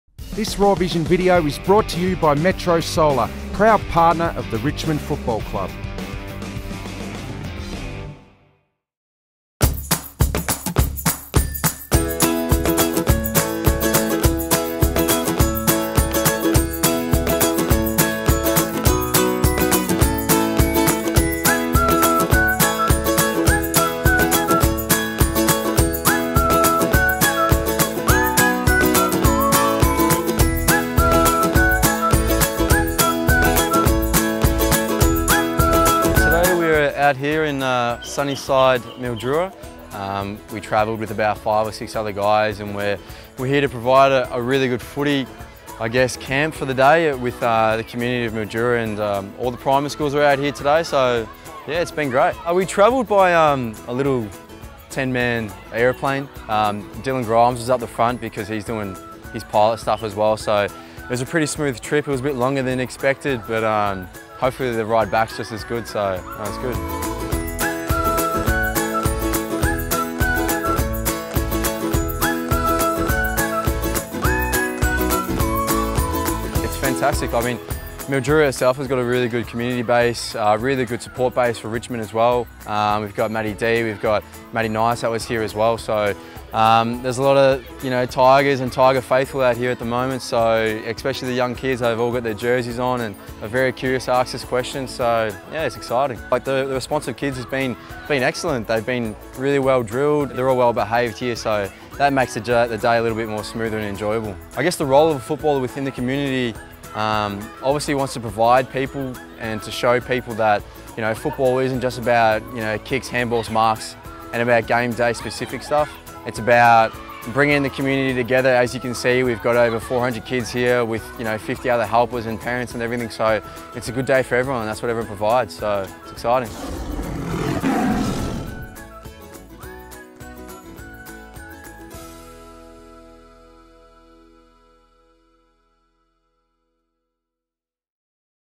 Airborne Tigers
Matt Arnot talks about the Tigers' Mildura Community Clinic.